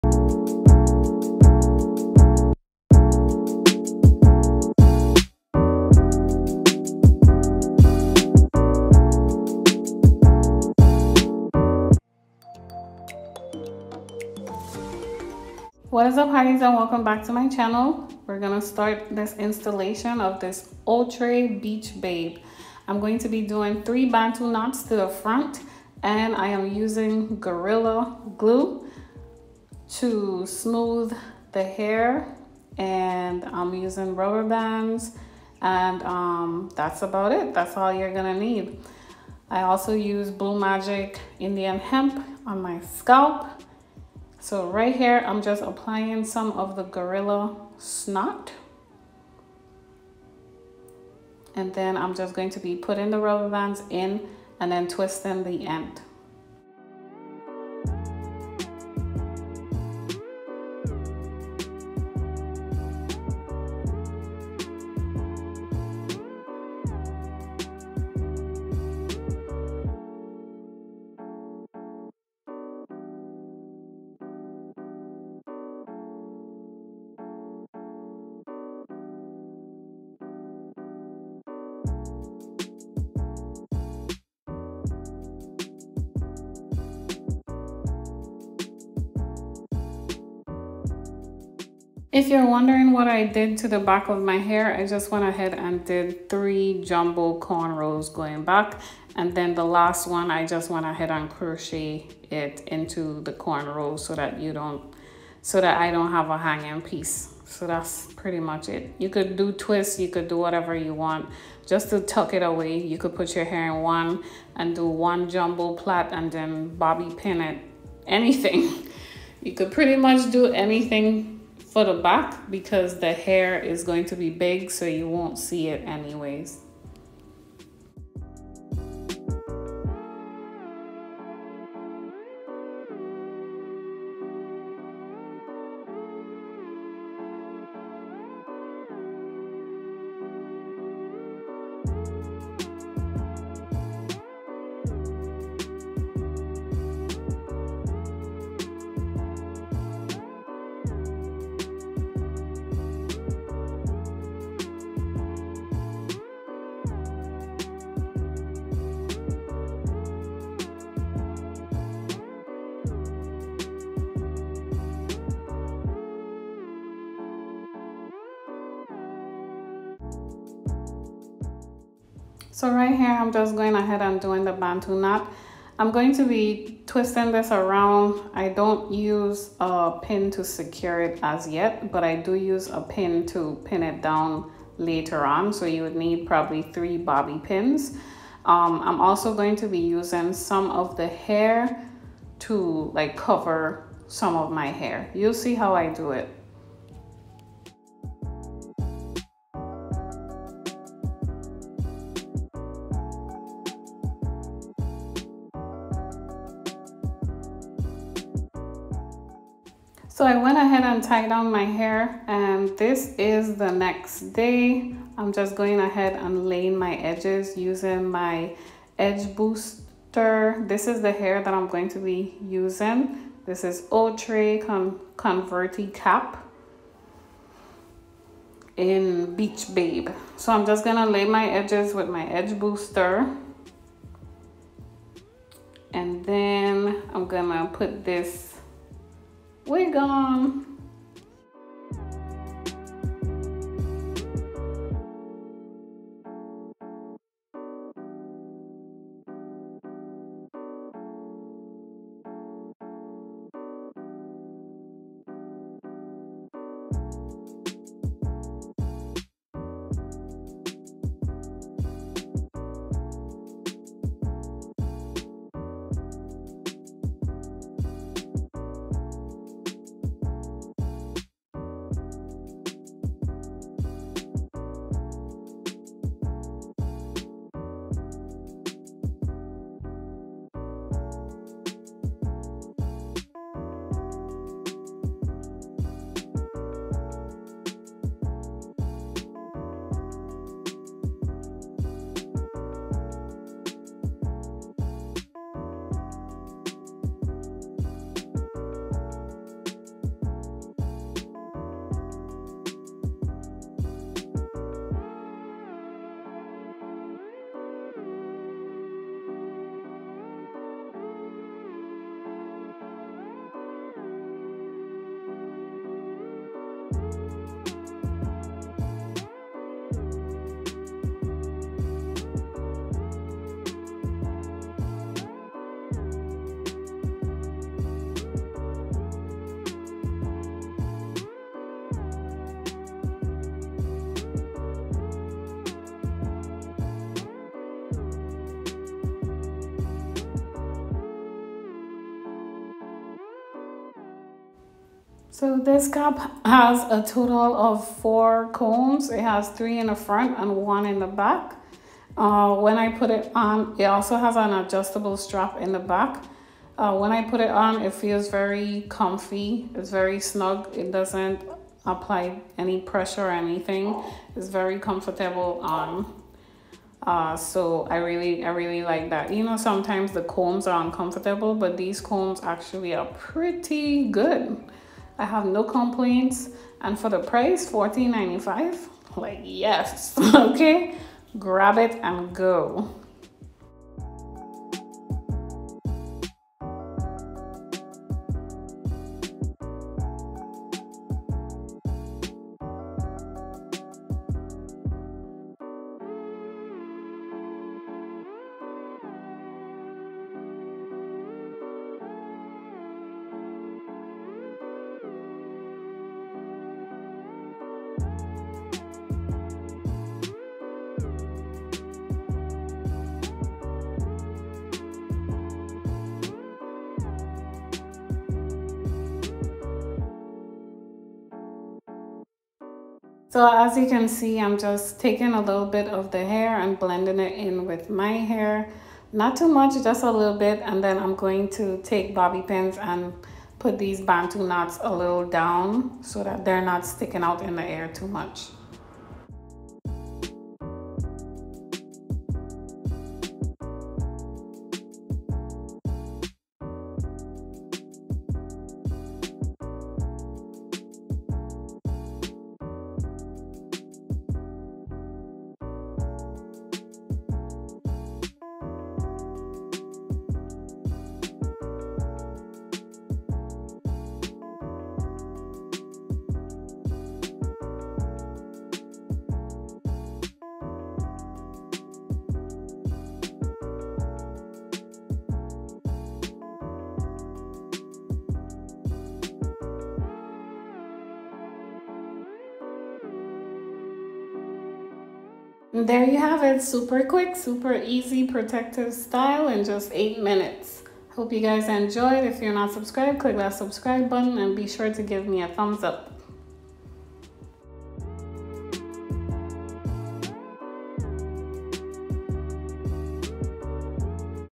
EASY 8 MIN BANTU KNOT CURLY NATURAL HAIR PROTECTIVE STYLE | Hair How To Outre: Beach Babe
Super cute curly protective do you can achieve in minutes. EASY 8 MIN BANTU KNOT CURLY NATURAL HAIR PROTECTIVE STYLE | Outre: Beach Babe Converti Cap

Do you love snacking, healthy snacking? Here's $20 worth of FREE SNACKS on me. And the prices are low! Thank me later.